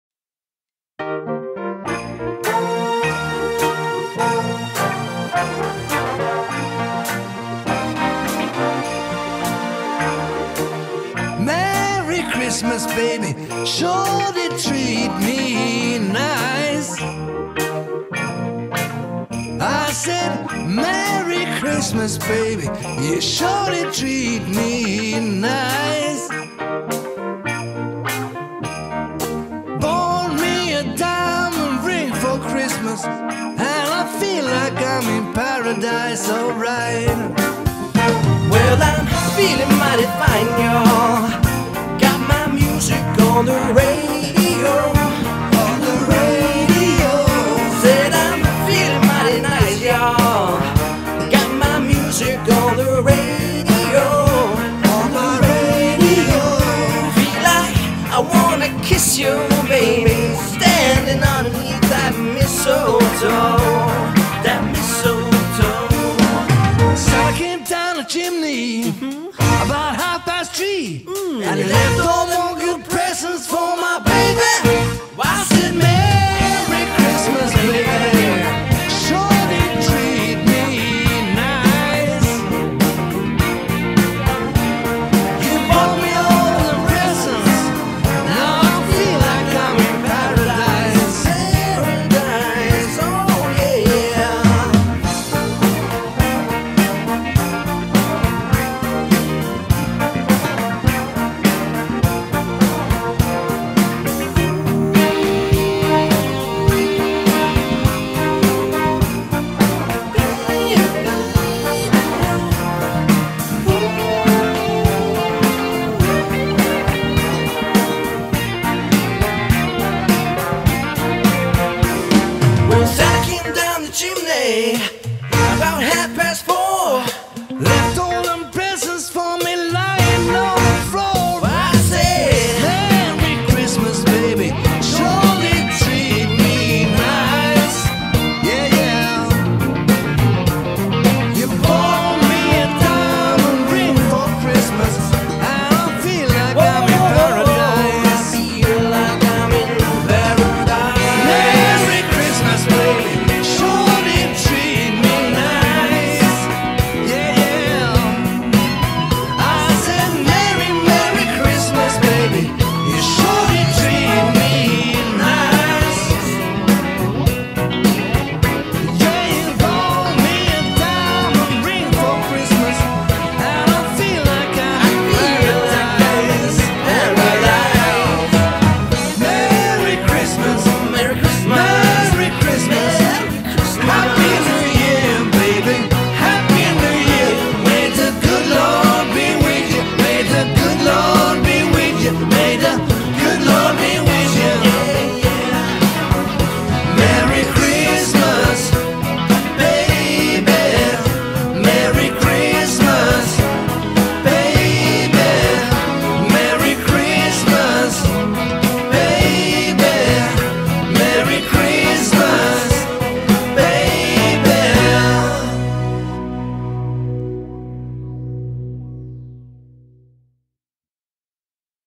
Julevideo 2017
Vocal: Frank Mosand and Henning Mosand Stephansen
Vadsø - Norway - December 2017